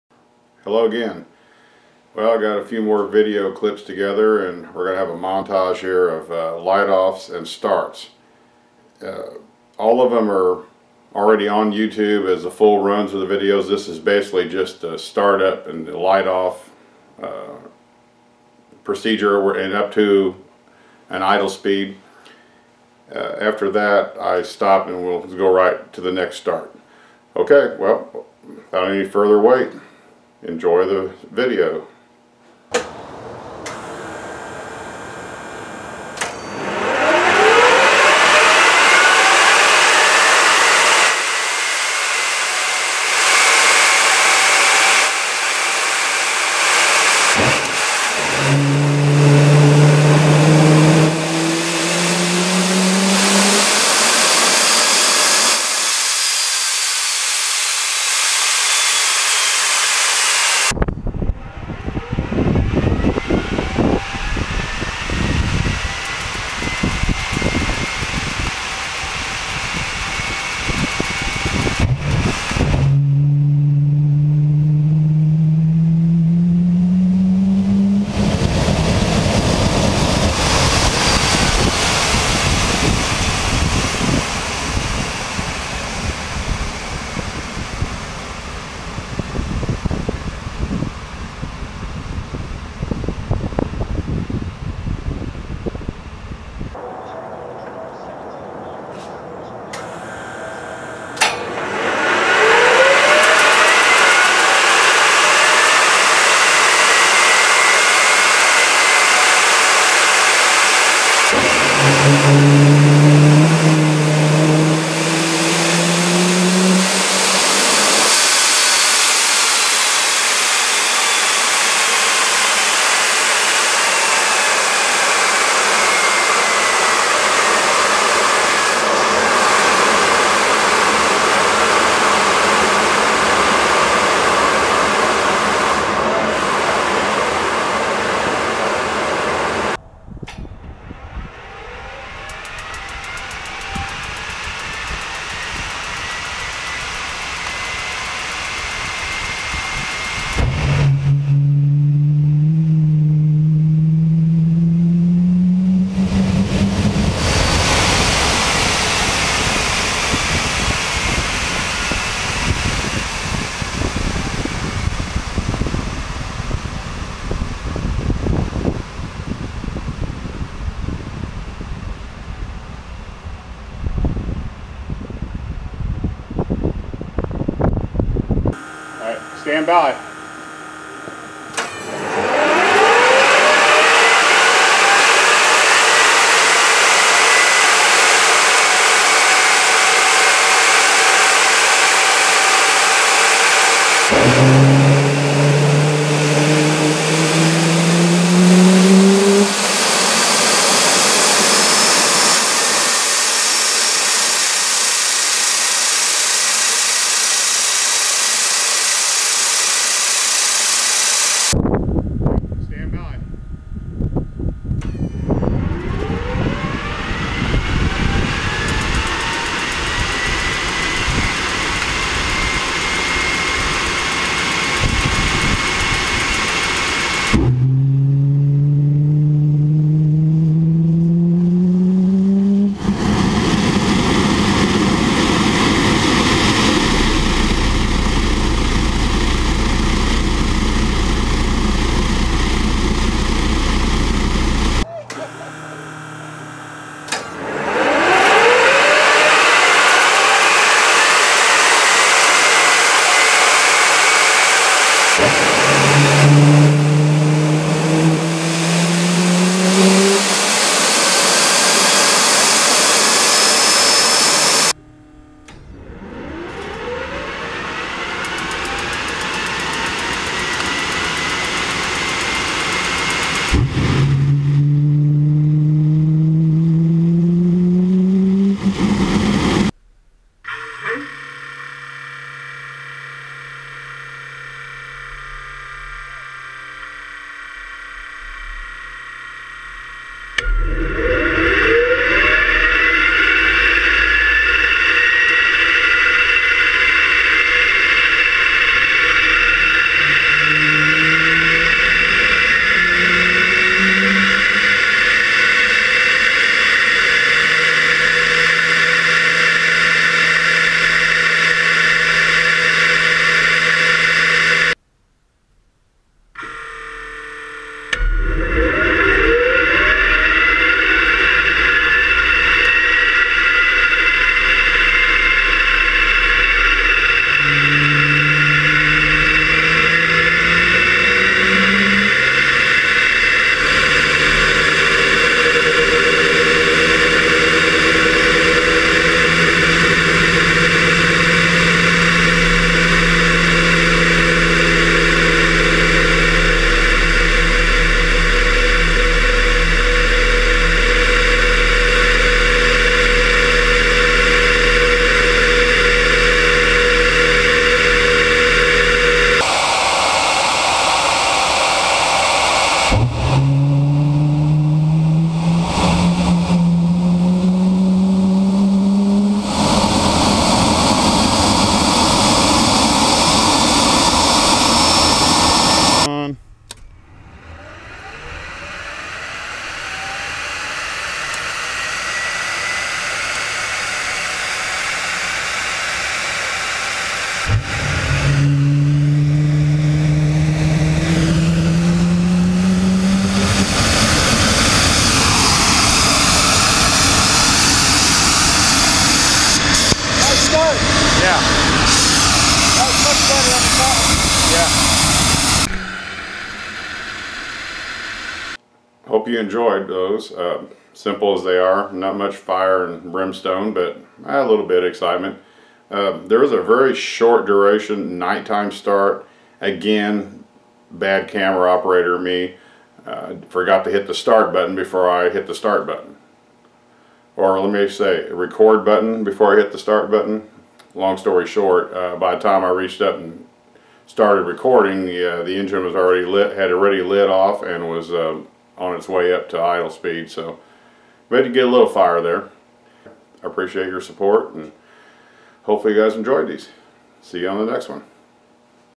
J44 Project Montage of Light Offs & Start Ups!
This video is a montage of just light-offs and starts!  At the end, there is a very short clip of a night start but I failed to start recording in time to catch it all!  Sorry.